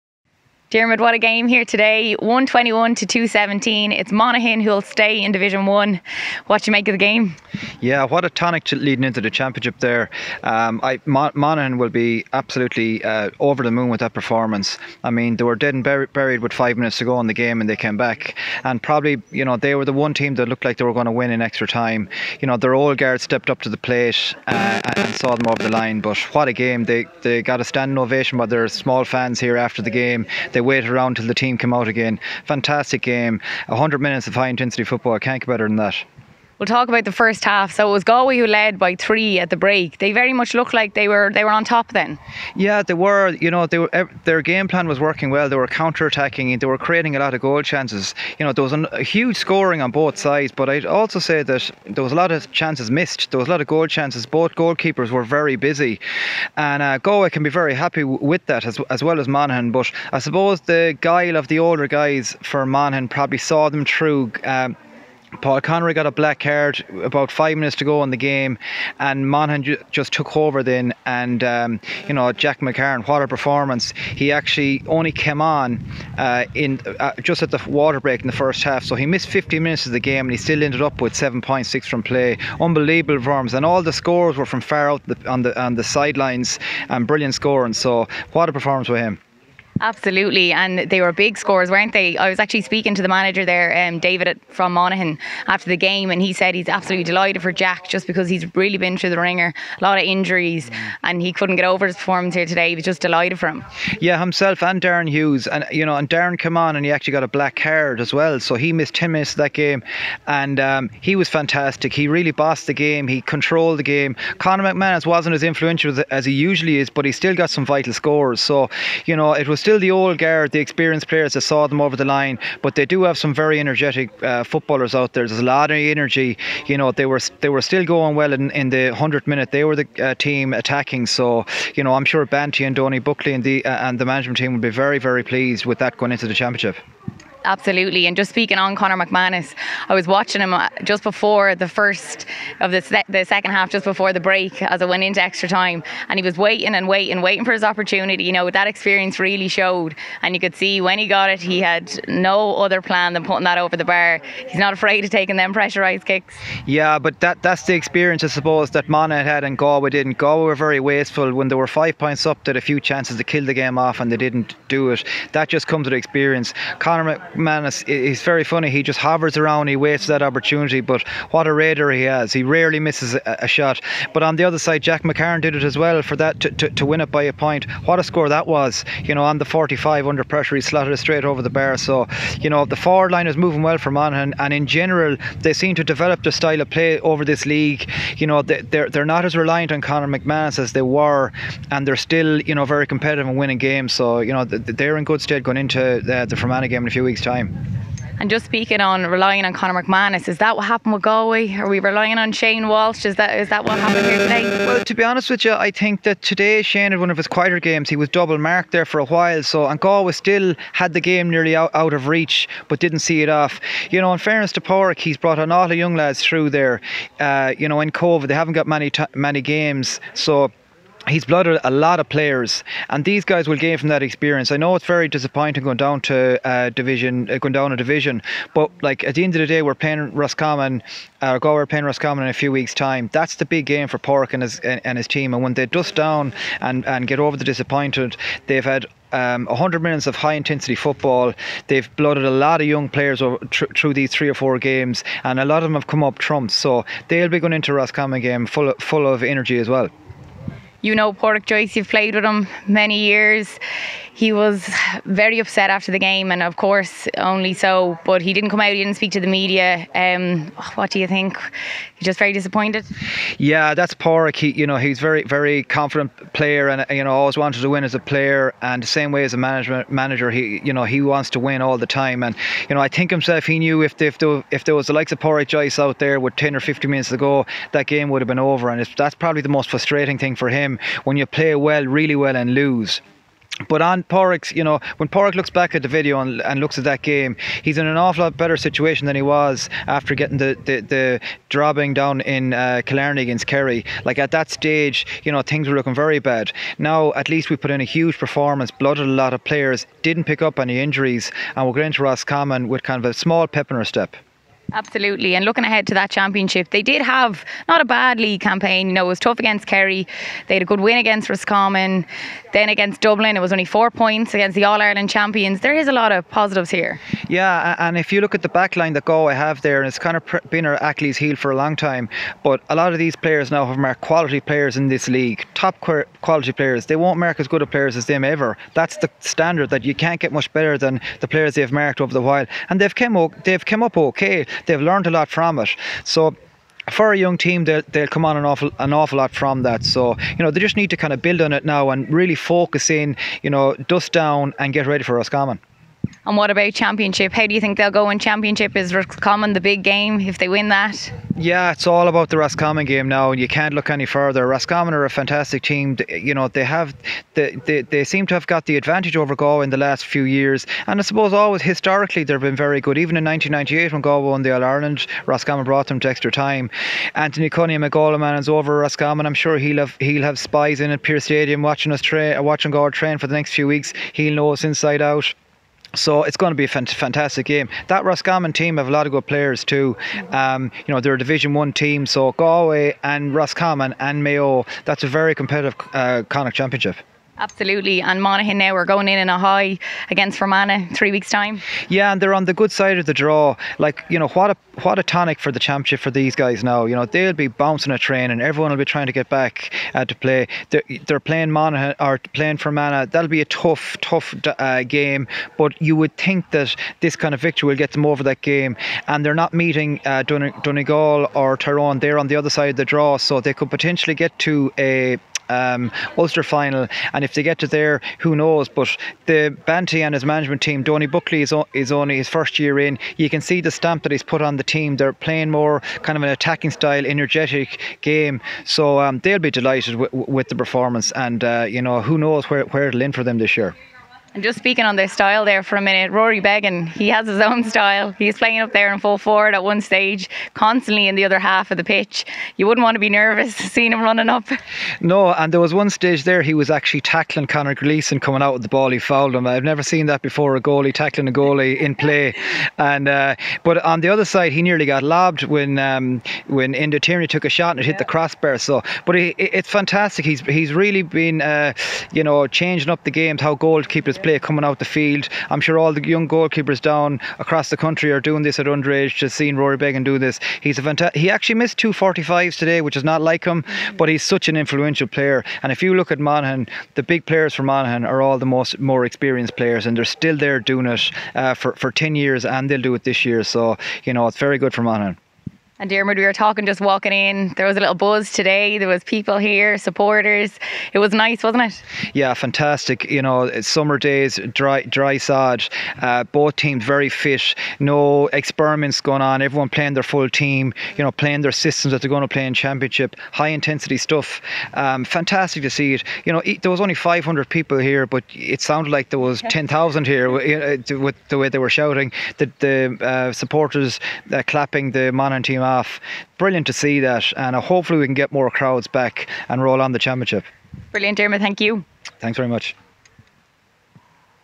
Diarmaid Blake | Monaghan stay in Division 1 | McManus the difference | Supporters are back
Former Galway footballer Diarmaid Blake joined Aisling O'Reilly to discuss Monaghan's 1-21 to 2-17 victory over Galway on Sunday.

They discussed the implications for Division 1 football, how Conor McManus was the difference for Monaghan, Galway not having the same experience on the pitch, the upcoming Championship, and the importance of having supporters back.

Have you downloaded the brand new OTB SPORTS app? It's got everything you need!

- Snapchat: add us and message the show @offtheball1